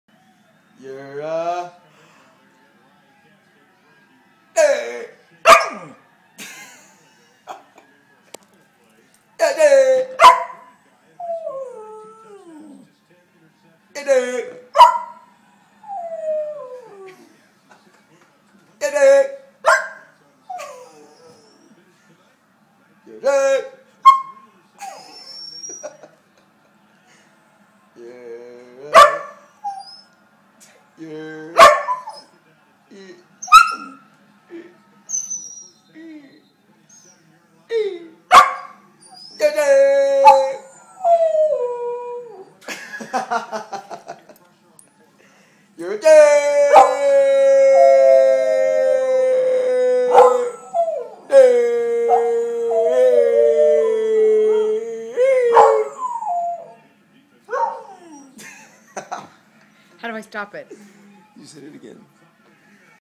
Howling Corgi
Our Corgi, Yogi does not like being  called Jerk!